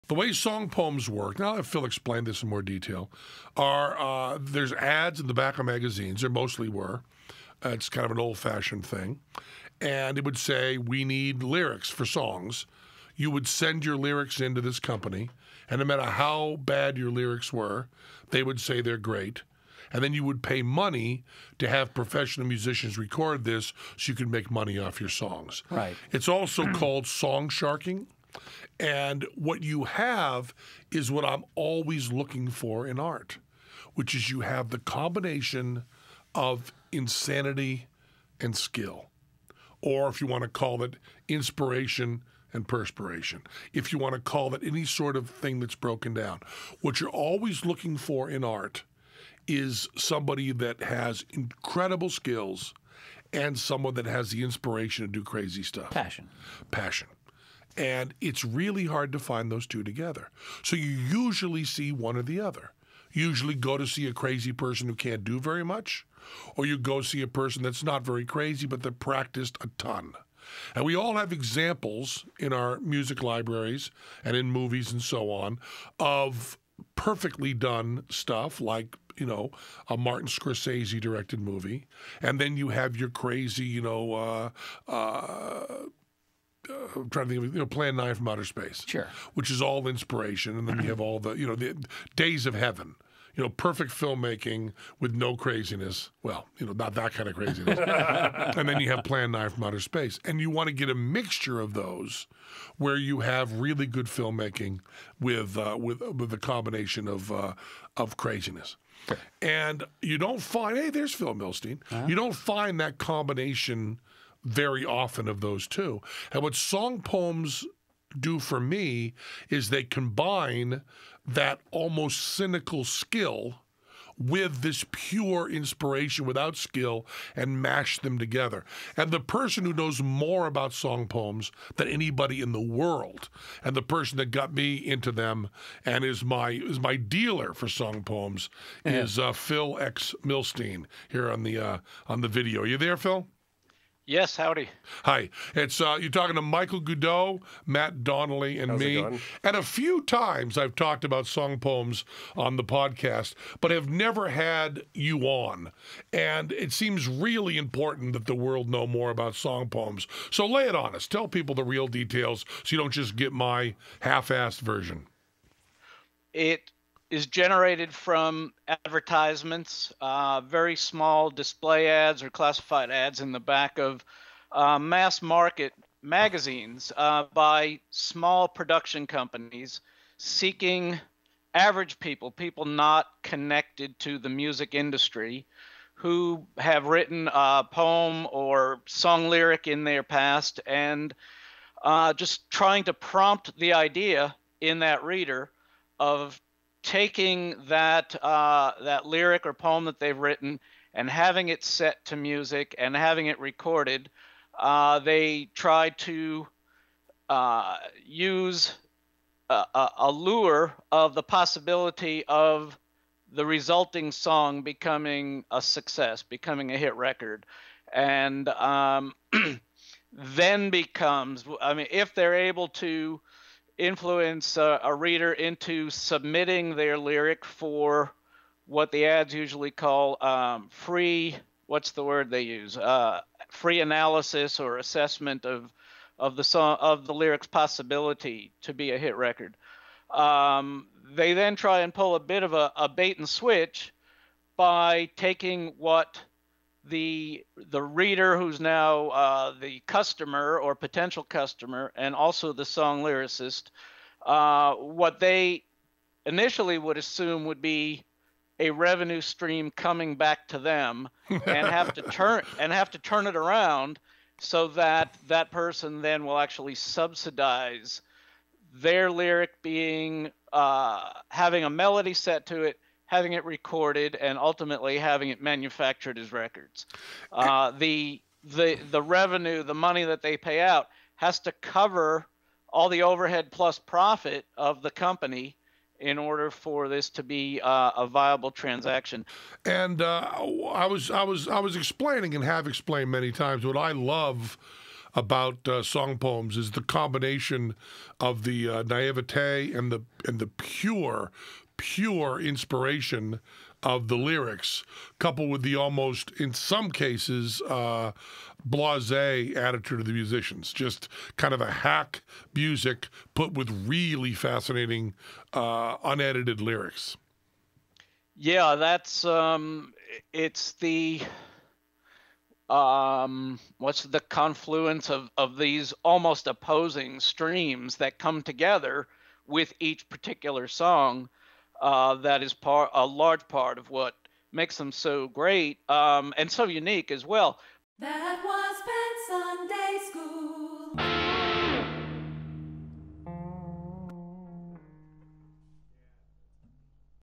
What are song poems?
Phil Milstein & Penn explain what song poems are & how they make money.